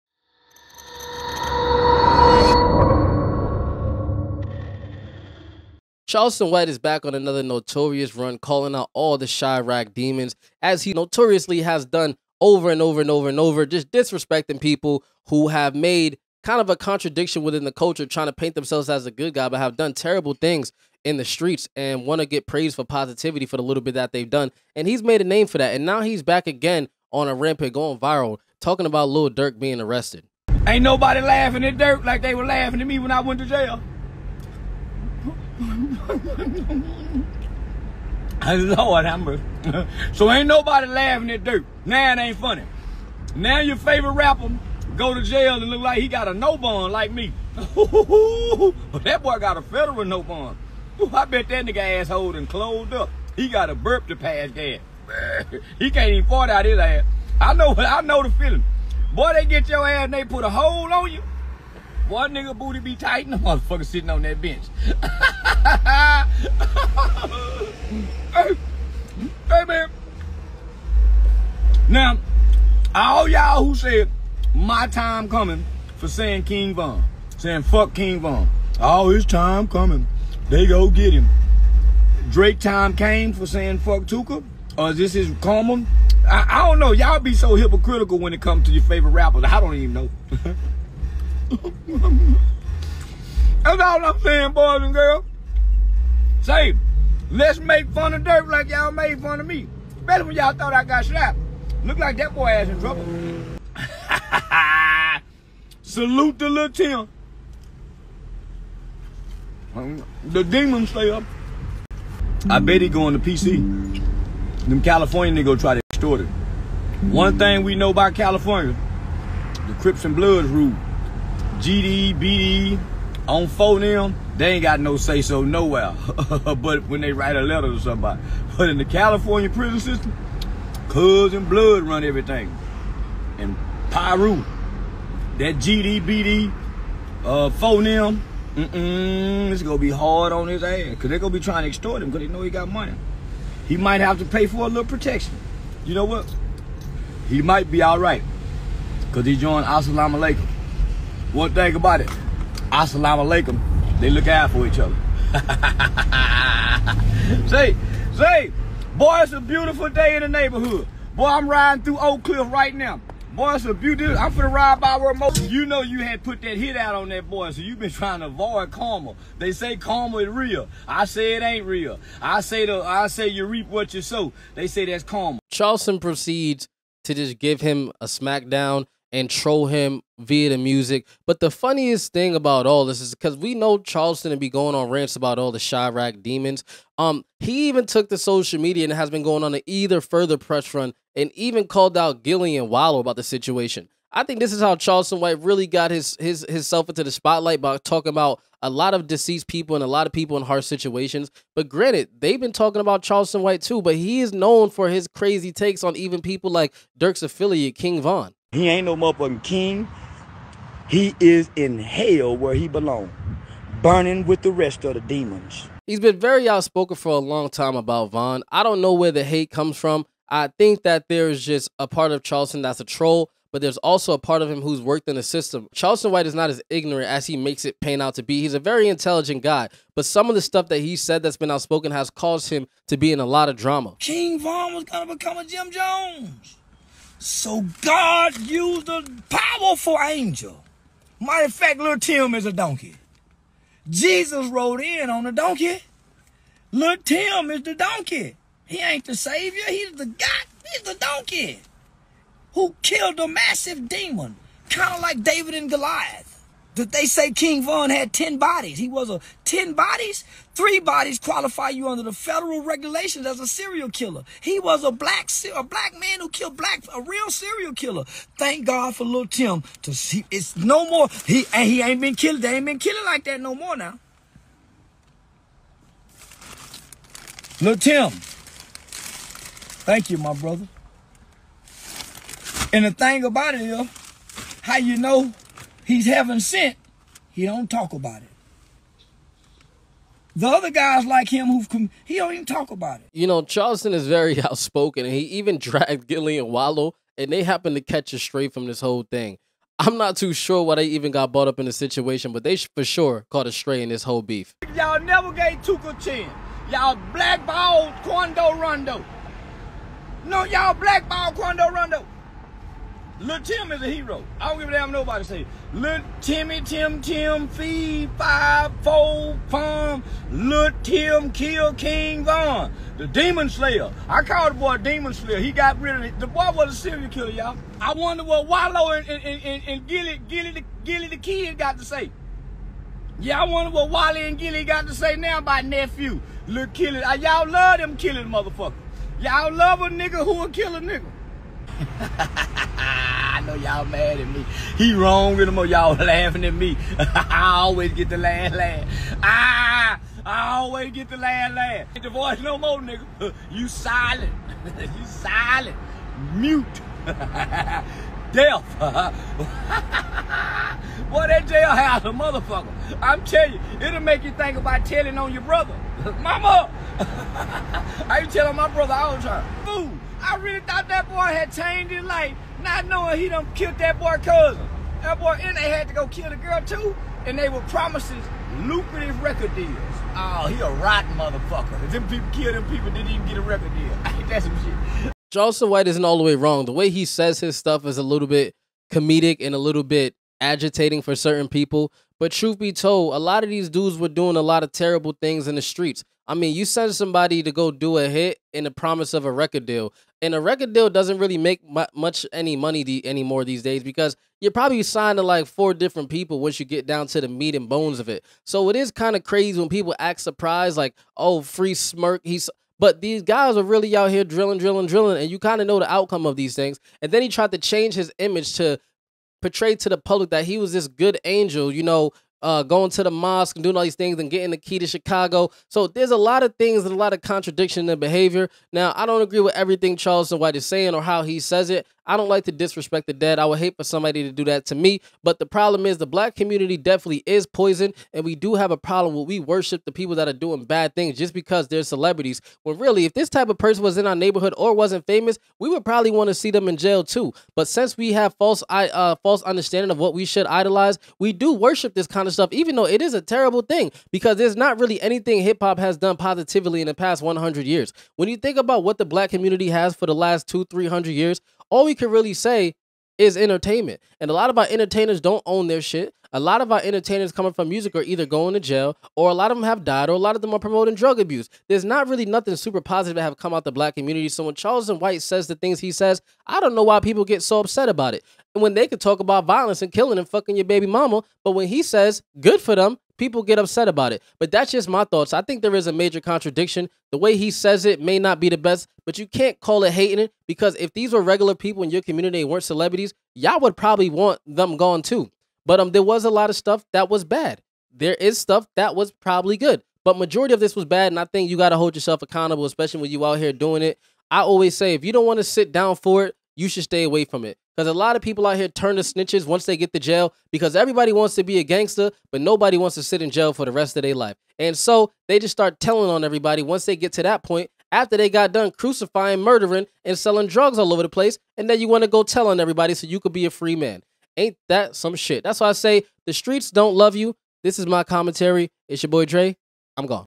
'YOUR BOOTY IS TIGHT' Charleston White GOES OFF On Lil Durk Arrest - Calls Out Gillie & Wallo
Charleston White slams Lil Durk in a fiery rant, calling out Gillie & Wallo. Get the scoop on the latest drama in this intense video!